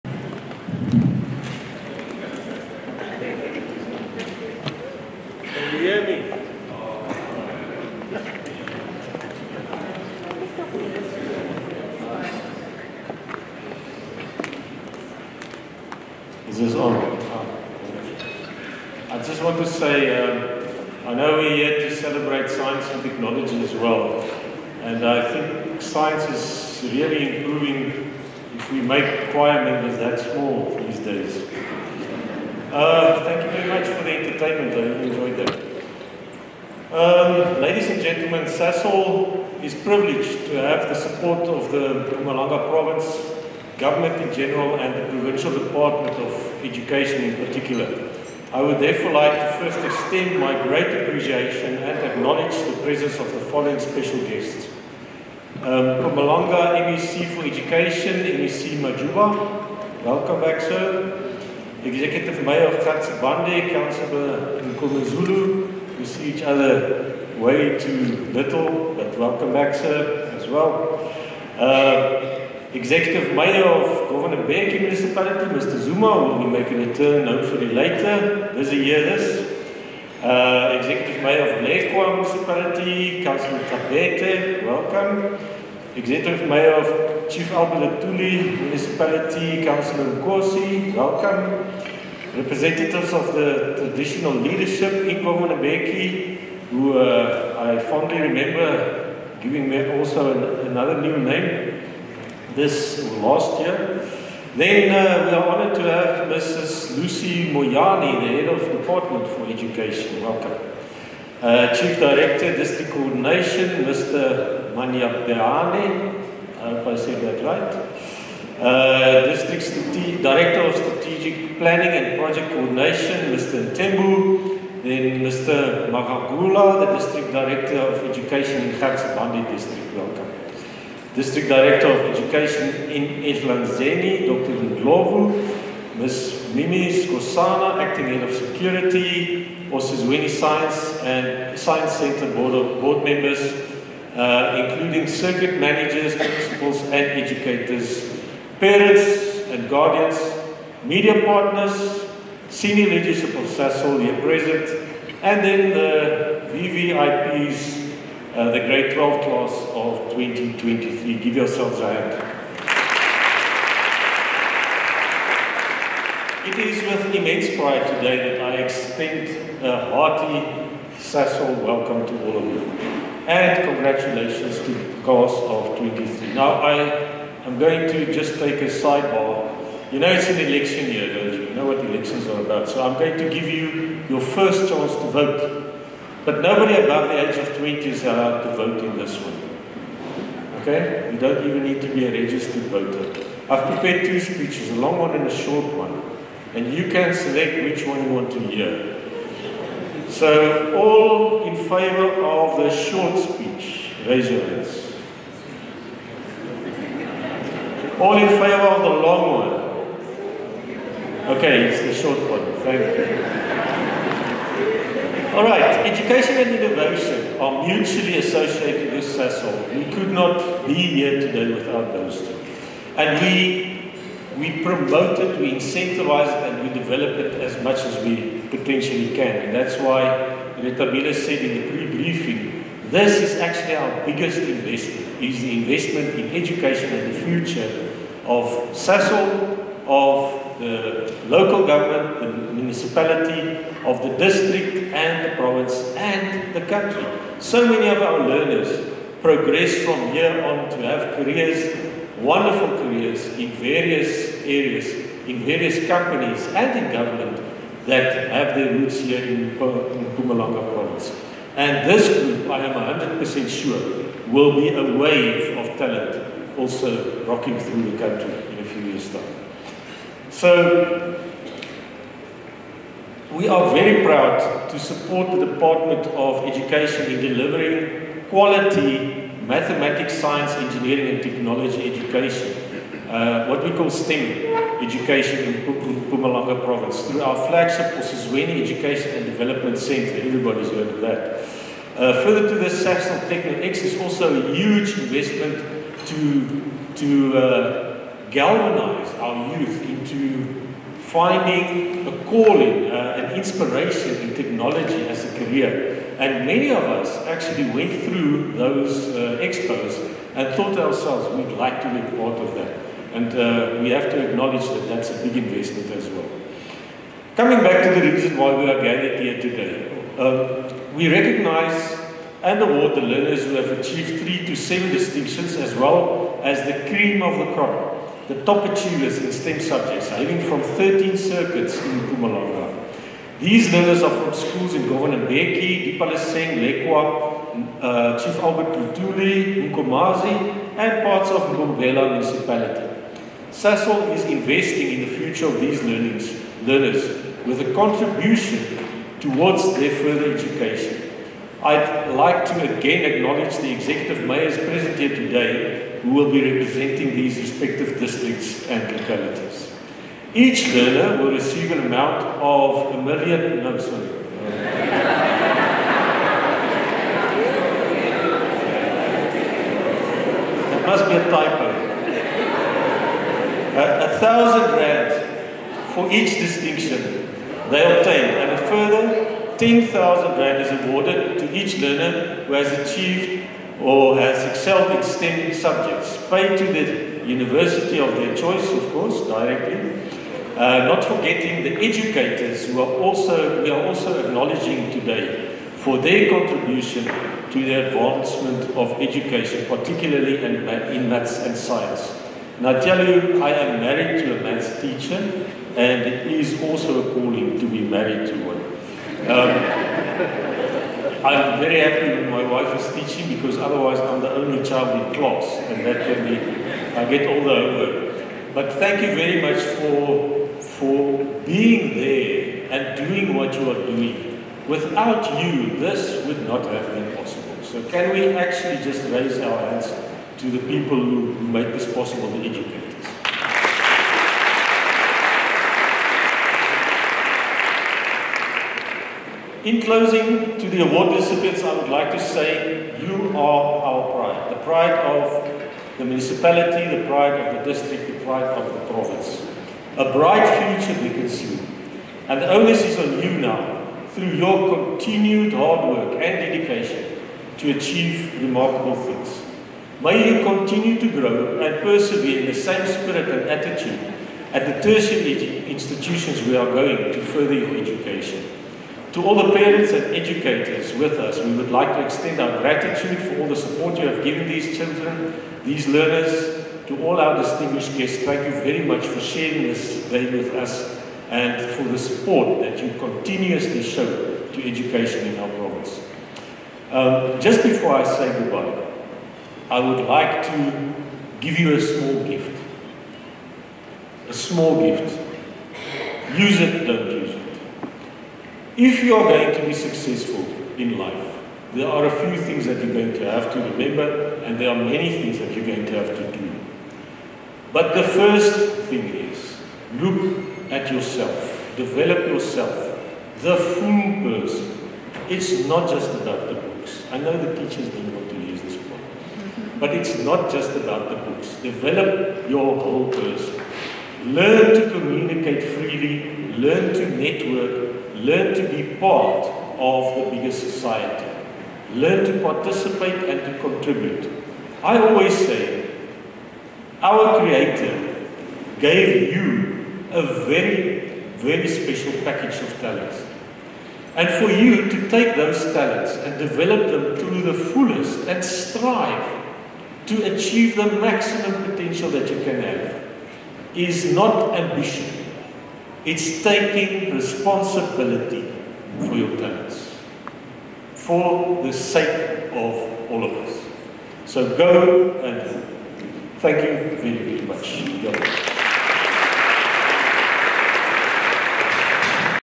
Gerrit Viljoen
On Thursday, 1 February 2023, Sasol hosted the 29th Grade 12 Merit Awards in Secunda to honour outstanding performers from the Class of 2023.
The awards are an annual initiative that aims to recognise, celebrate and rewards Grade 12 top achievers as well as encourage learners to enrol in Science, Technology, Engineering and Mathematics (STEM).
To honour the role played by educators, the best-performing subject teachers and best-performing schools were also recognised. More than 130 awards handed out during the ceremony.